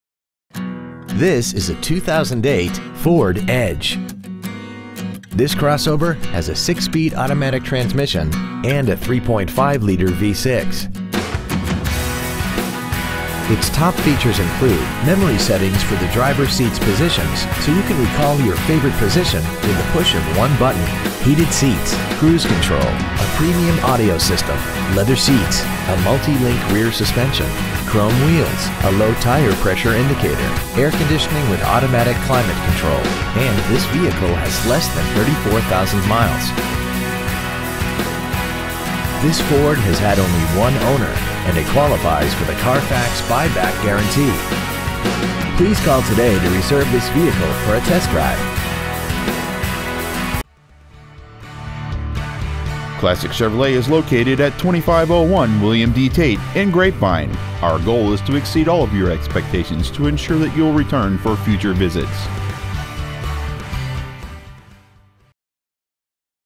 Pre-Owned 2008 Ford Edge Ft. Worth TX
Year: 2008
 Make: Ford
 Model: Edge
 Engine: Gas V6 3.5L/213
 Trans.: Automatic
 Exterior: Pearl White
 Miles: 33,585
 Interior: Tan

 2008 Ford Edge Grapevine TX